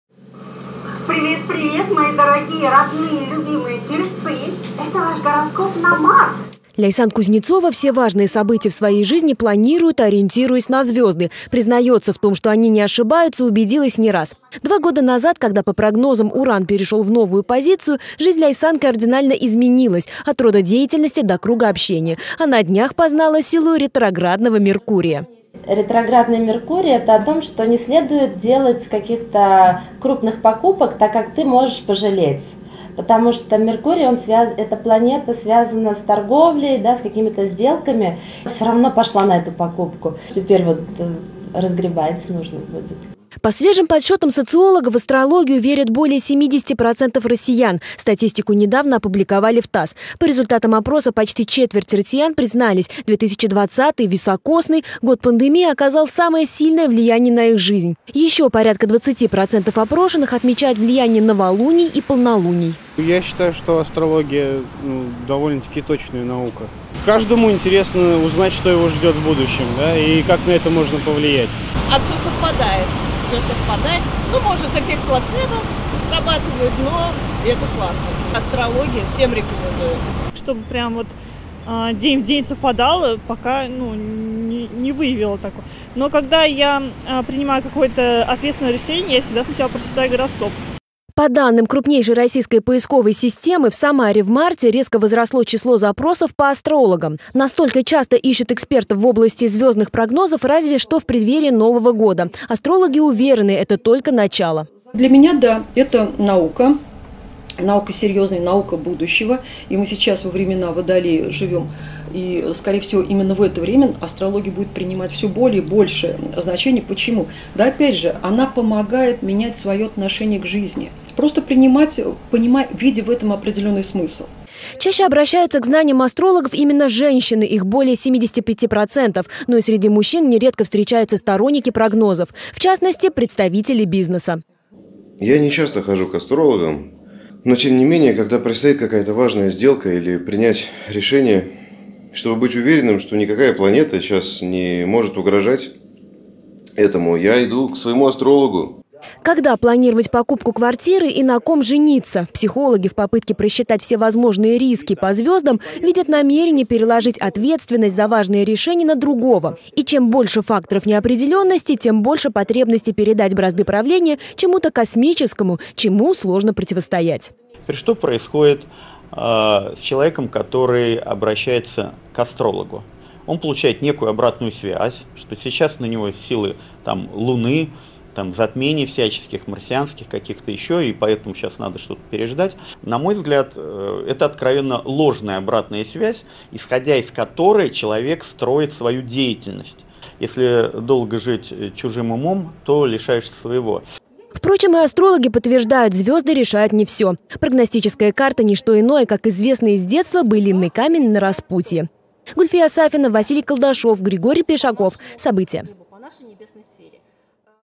Почему люди верят предсказаниям астрологов
Желают знать, что будет. Астрологию, как неофициальную, но все же науку все больше россиян сегодня воспринимают всерьез. По результатам февральских исследований, в гороскопы верят 71% жителей России. Одни интересуются звездами, когда предстоит важное событие, другие в вовсе все решения принимают исходя из прогнозов астрологов. Гульфия Сафина попробовала разобраться в теме и узнать точки зрения двух сторон: тех, кто верит и тех, кто не верит в астрологию вовсе.